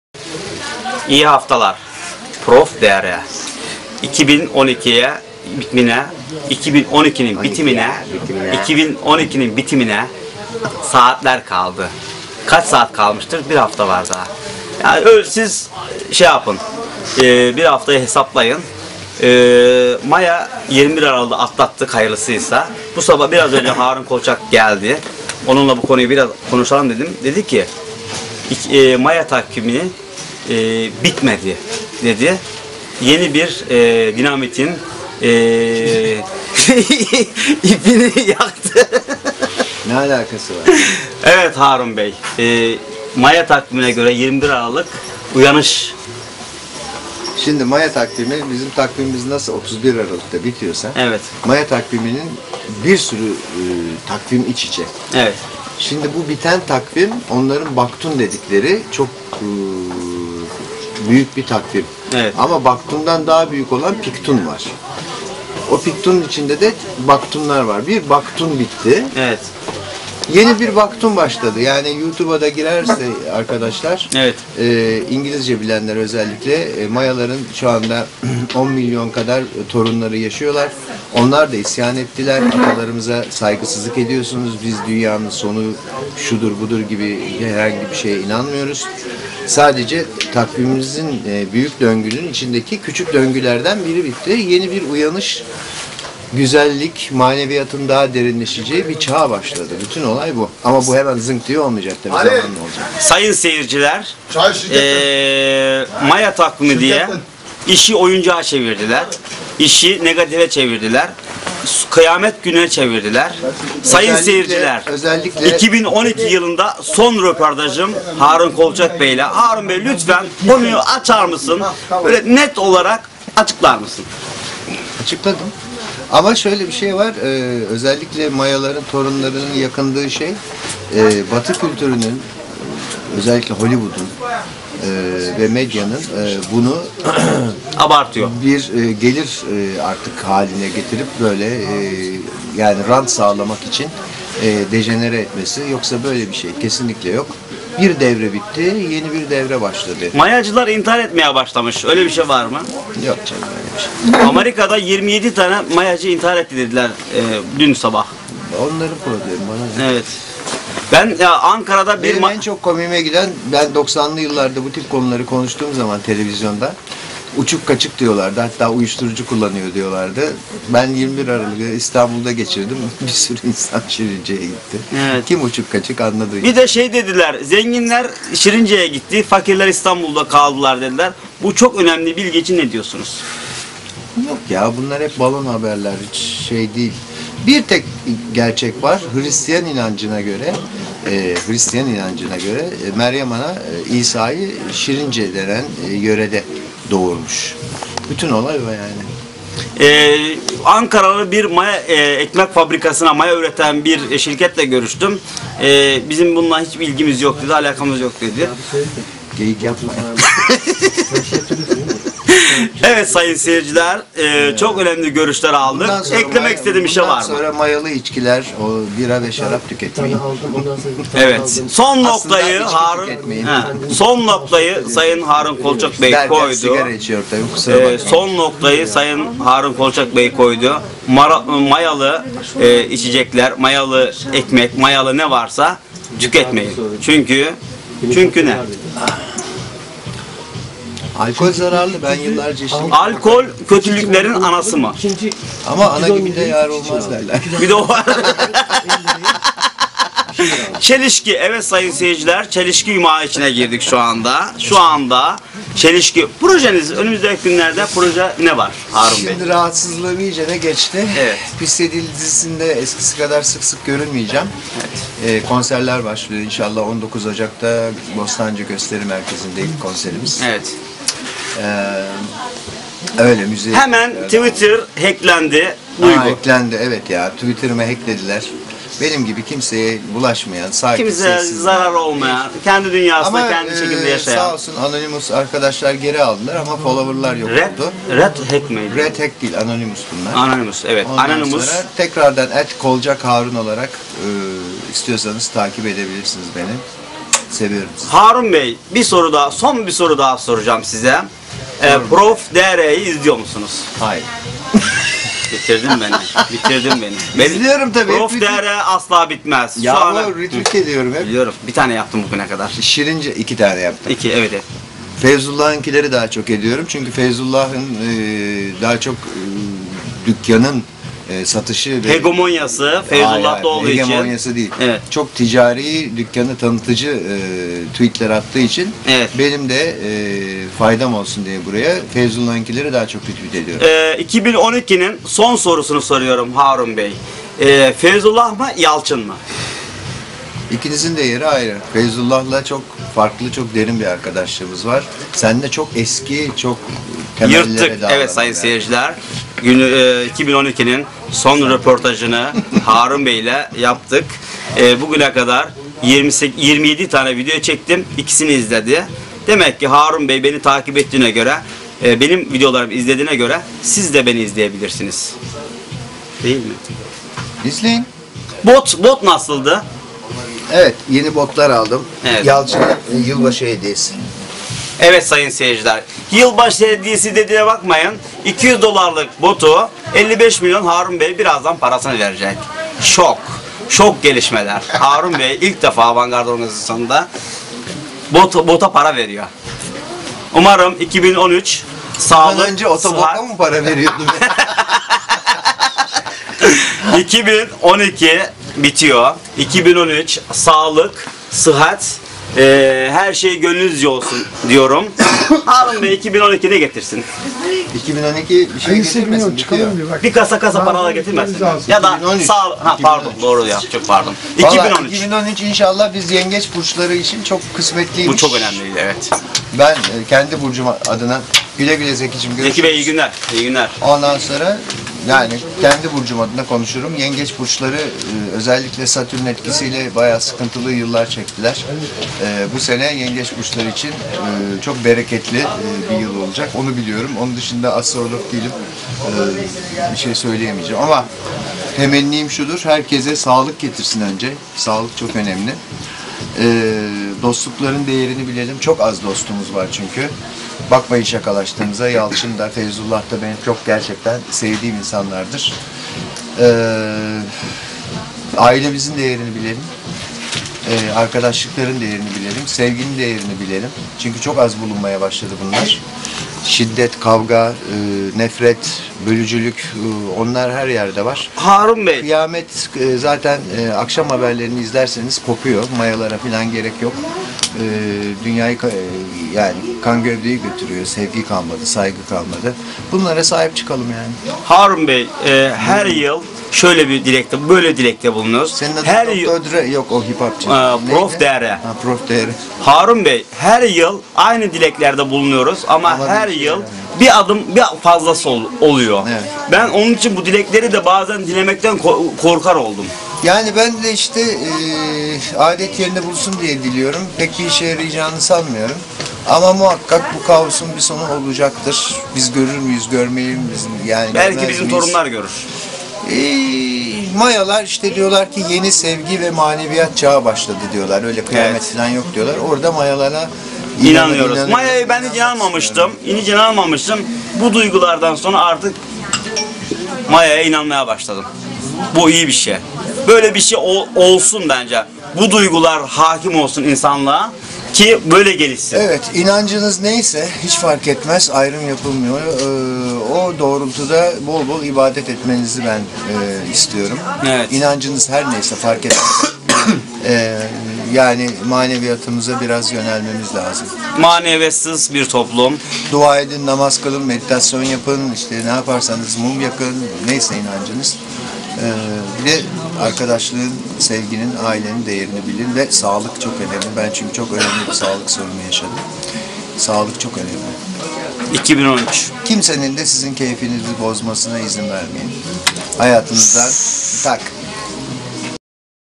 Sevgili Dostum Harun kolçakla cok eğlendiğim bir video
Yalçın Avcı ve Harun Kolçak Mayalar Hakında bilinmeyenler Sondakika 2014

Tüm sosyal paylaşim sitelelerinde

Gurmeyoutuber

Eleştiri ve yeni fikirlere acığiz
Teşekkürler